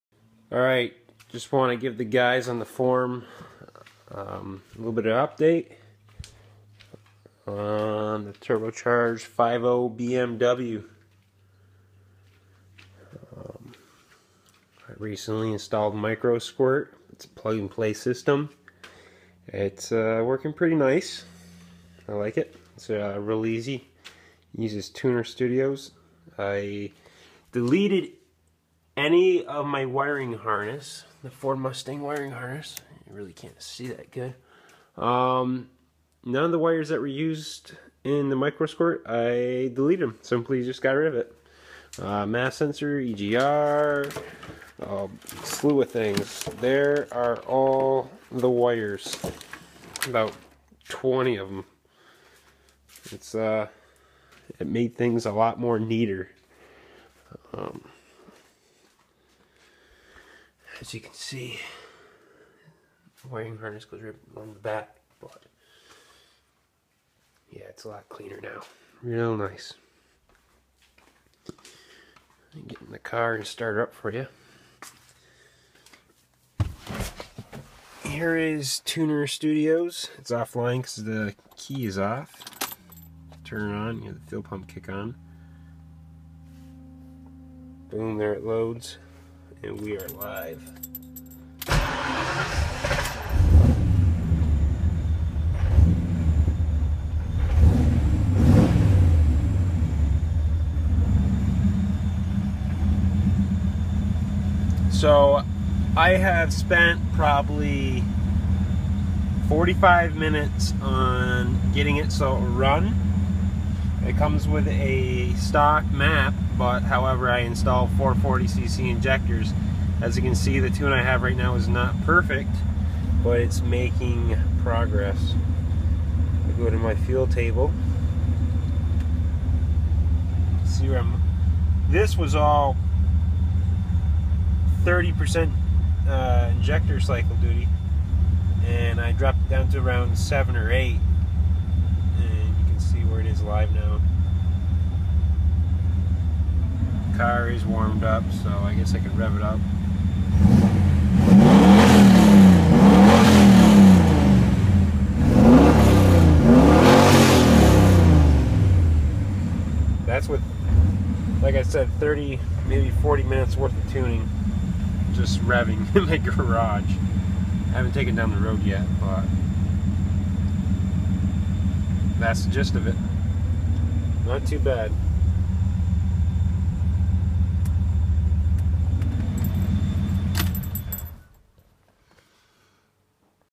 Turbo 5.0 e36 M3 microsquirt
Just a short video of my turbo 5.0 m3 on microsquirt.